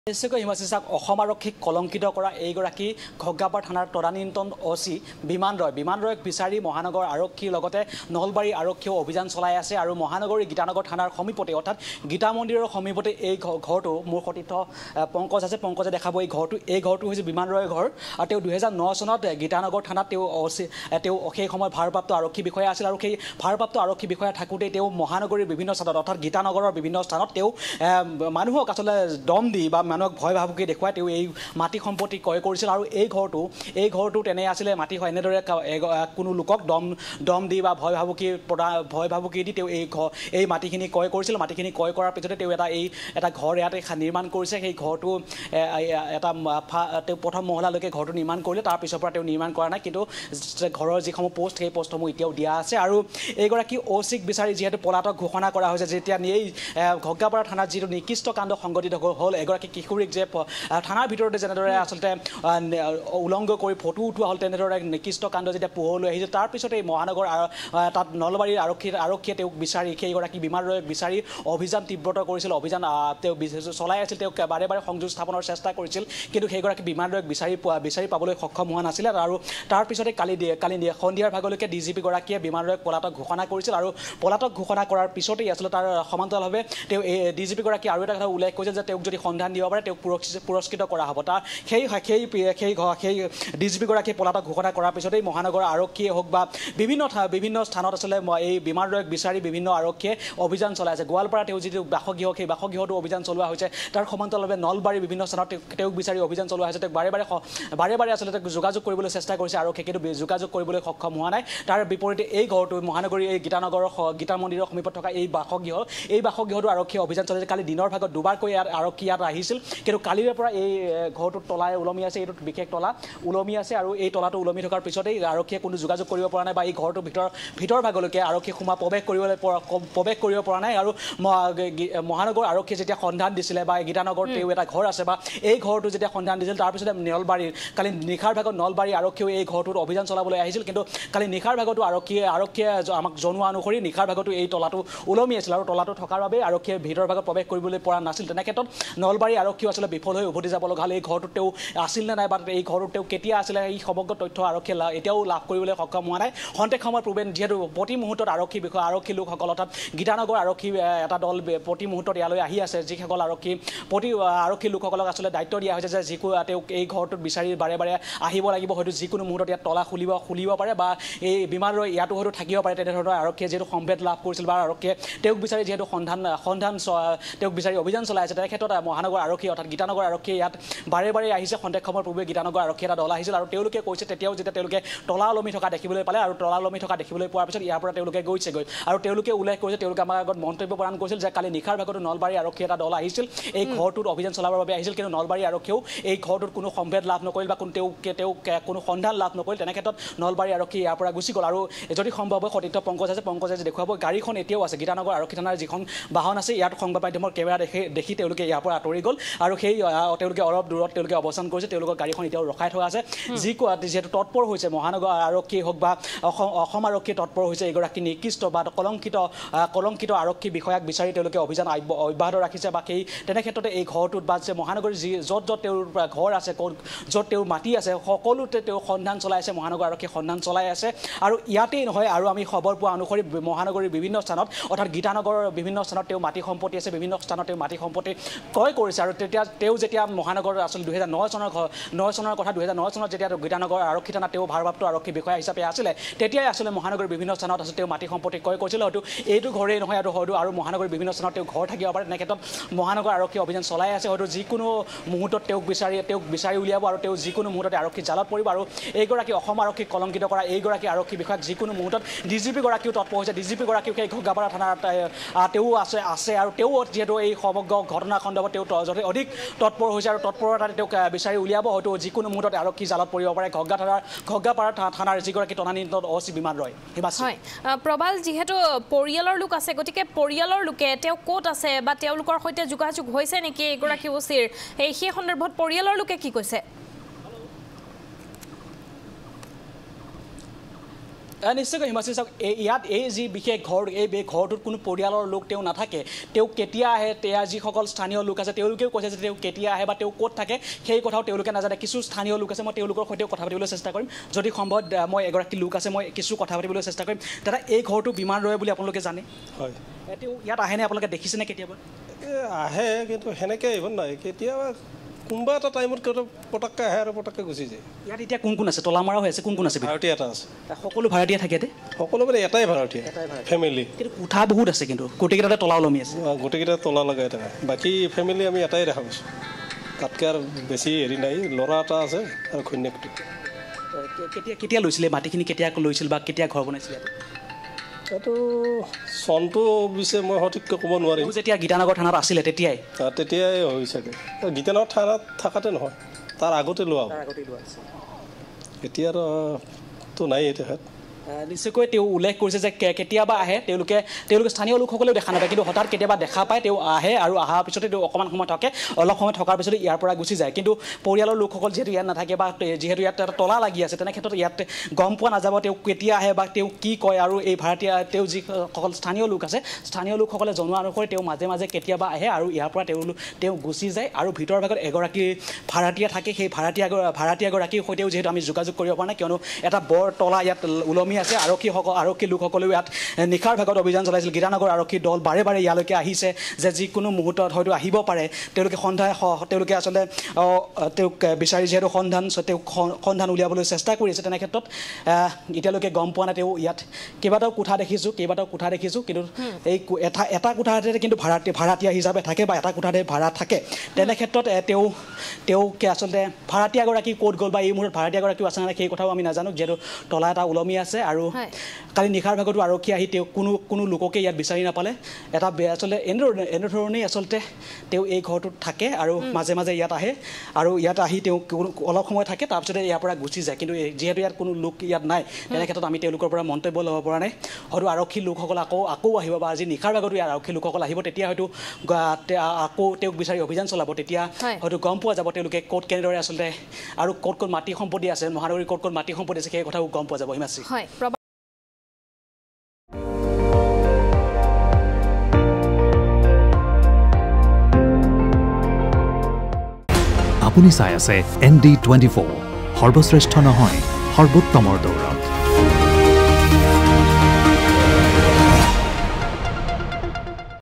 গীতা মন্দিৰৰ সমীপত বিমান ৰয়ৰ ঘৰত আৰক্ষীৰ অভিযান
ক’ত আছে পলাতক বিমান ৰয়? গীতা মন্দিৰৰ সমীপত বিমান ৰয়ৰ ঘৰত আৰক্ষীৰ অভিযান। পুৰস্কাৰ ঘোষণা কৰিও বৰ্তমানলৈকে সন্ধান পোৱা নাই ঘগ্ৰাপাৰ থানা কাণ্ডৰ খলনায়ক বিমান ৰয়ৰ।